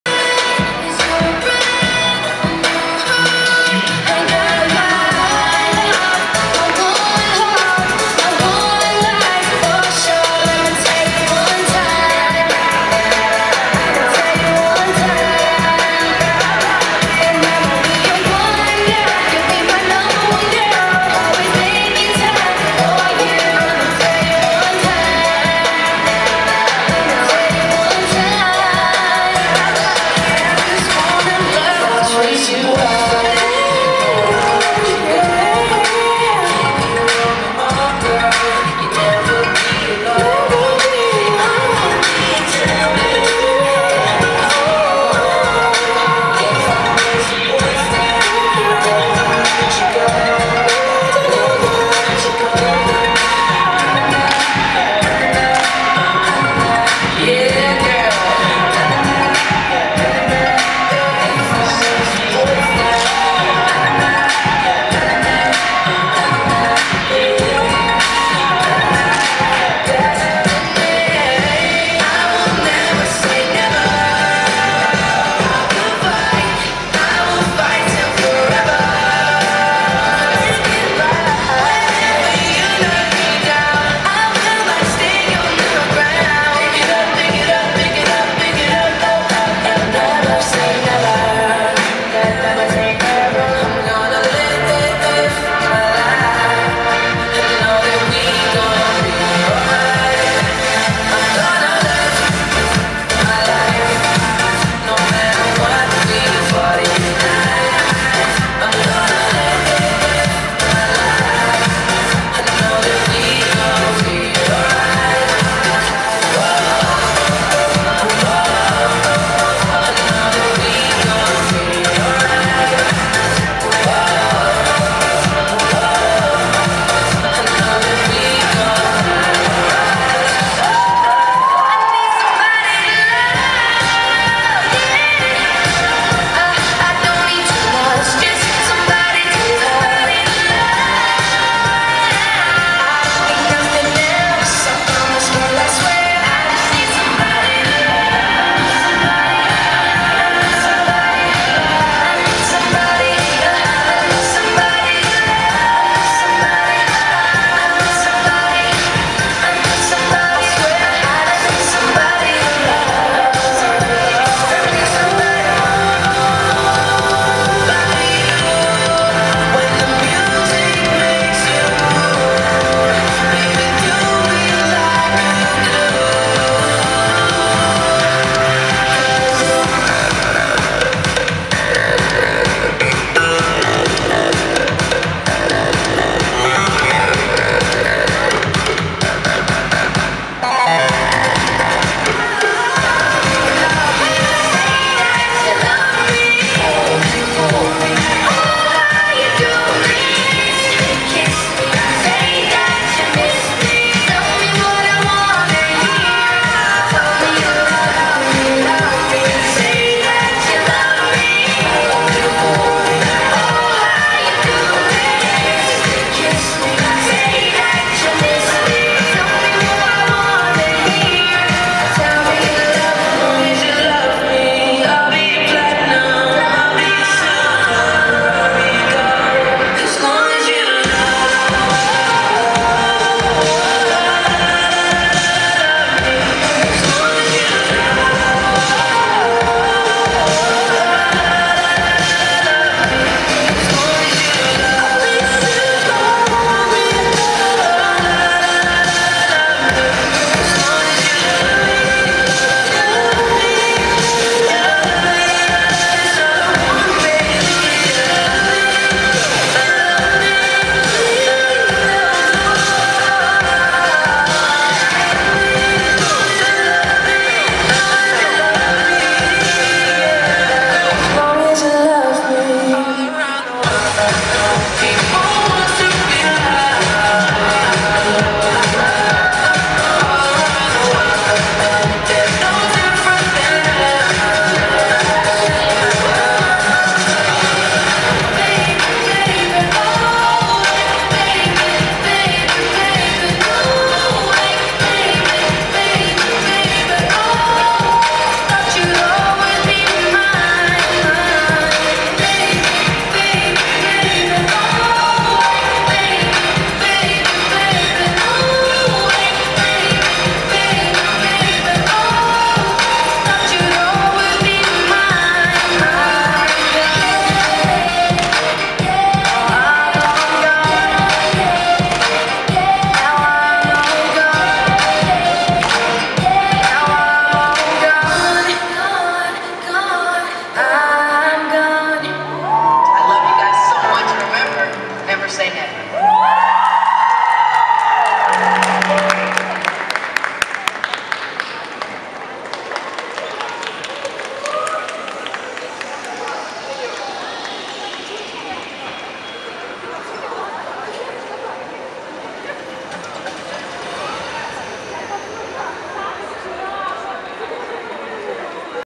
Russian flashmob by beliebers in PITERLAND 20.04.13
We love you Bieber :)